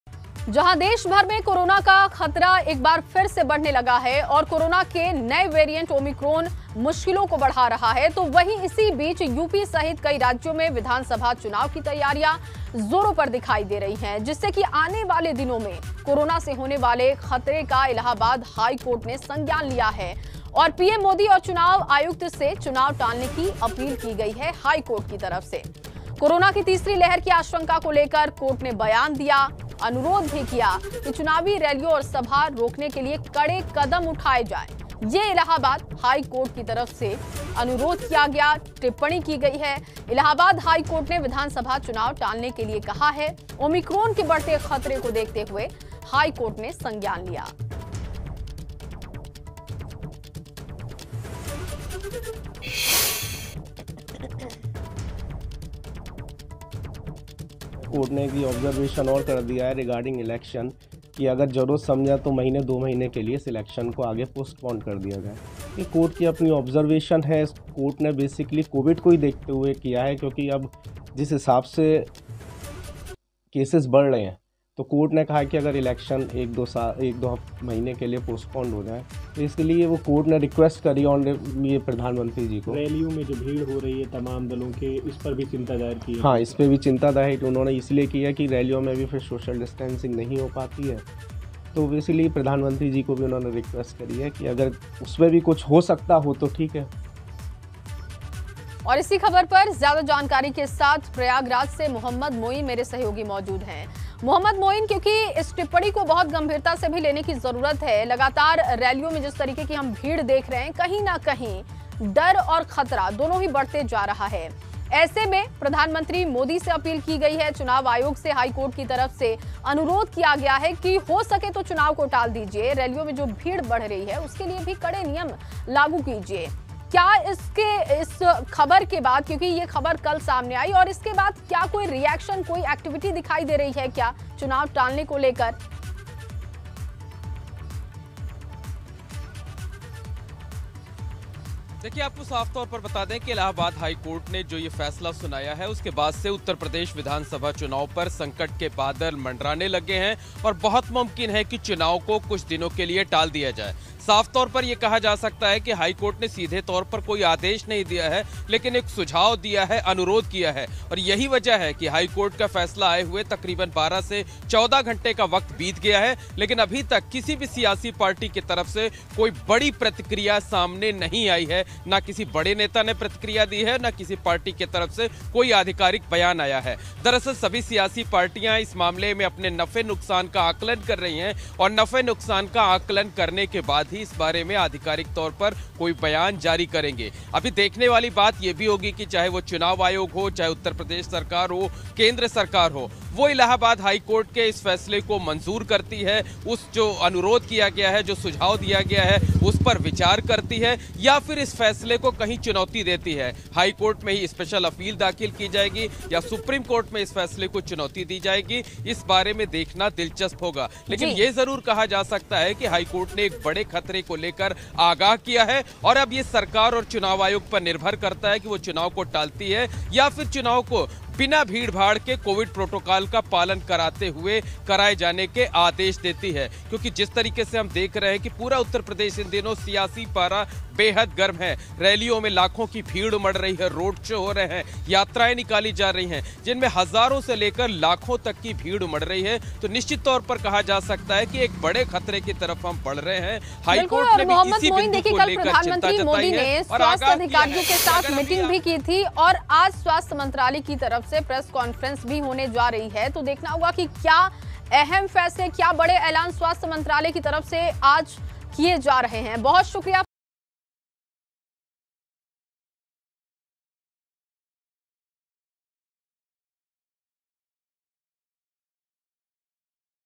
Omicron Sacre: इलाहबाद हाईकोर्ट ने की चुनाव टालने की अपील, क्या लगेगा चुनावी शोर पर ब्रेक?
जहां देशभर में कोरोना का खतरा लगातार बढ़ता जा रहा है और कोरोना के नए वेरिएंटओमीक्रॉन को लेकर लोग दहशत में है इसी बीच देश के 5 राज्यों में विधानसभा चुनाव को लेकर चुनावी तैयारियां जोरों शोरों पर है। कोरोना की तीसरी लहार की आशंका को देखते हुए इलाहाबाद हाई कोर्ट ने पीएम मोदी और चुनाव आयुक्त से अपील की है। इलाहाबाद हाई कोर्ट ने विधानसभा चुनाव टालने को कहा।